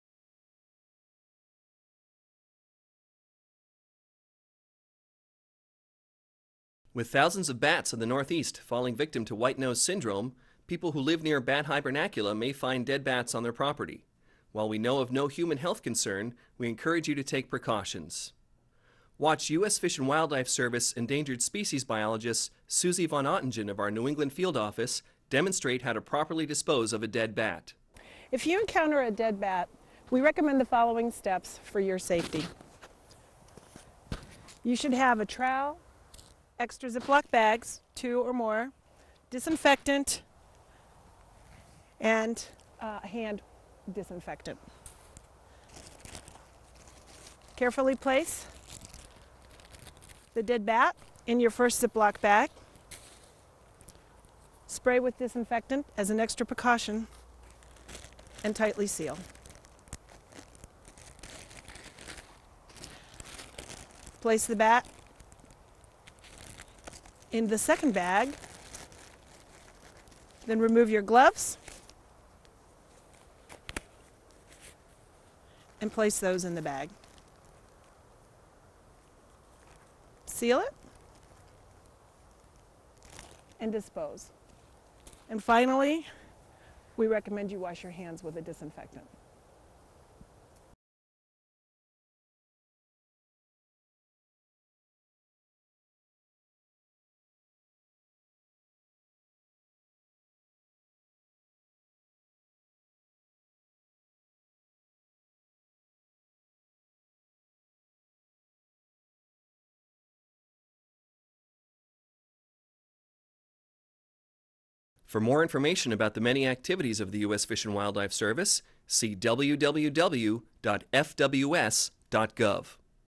How to Properly Dispose of a Dead Bat
U.S. Fish and Wildlife Service Biologist Susi von Oettingen demonstrates the proper and safe way to dispose of a a dead bat.